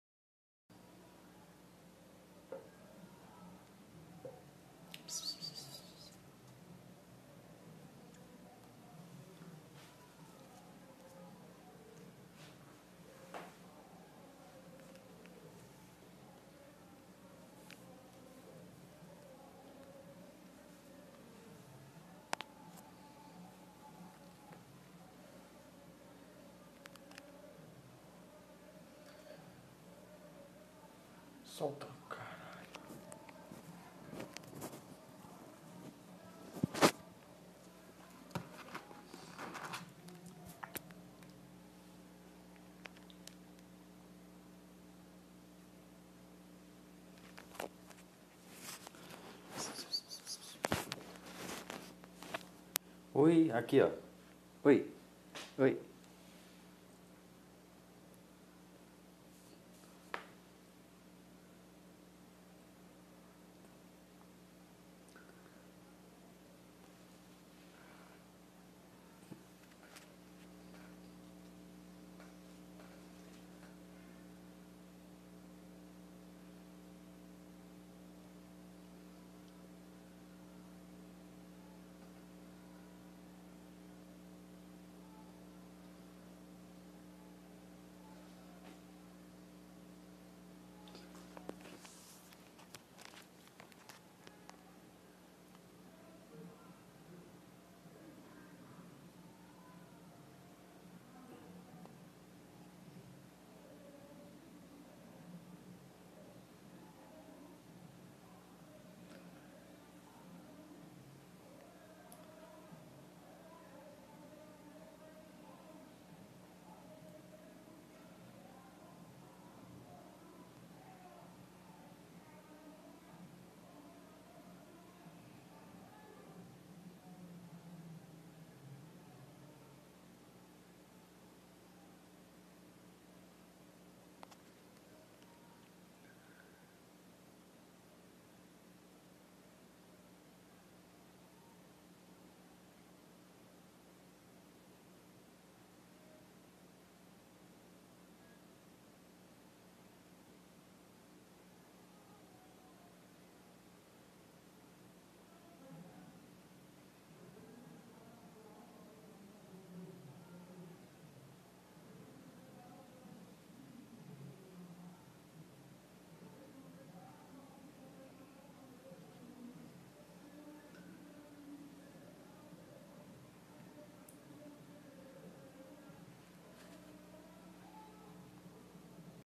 Esmeulon - Salvei Da Mãe ☠️Canibal
Vídeo de Esmeulon - Guitarrista ☠️🔥
Vídeos Feito A 3 Dias. // A Gata Come Os Filhotes Vivos. Salvei 2 Gatas Fêmeas.